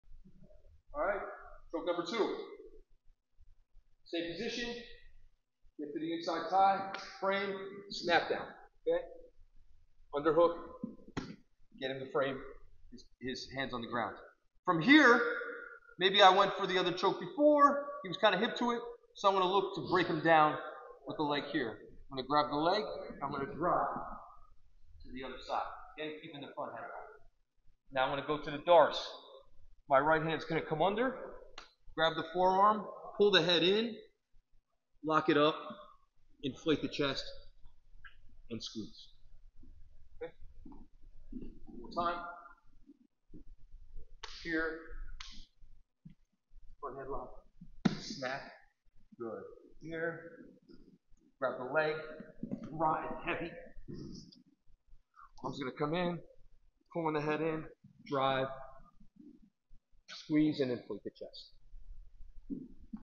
Chokes From Front Headlock - Part 2 - Darce
In this video, we reiterate using the Front Headlock to break our opponent down and then begin to attack the Darce.

Pay close attention to the details on the Darce finish.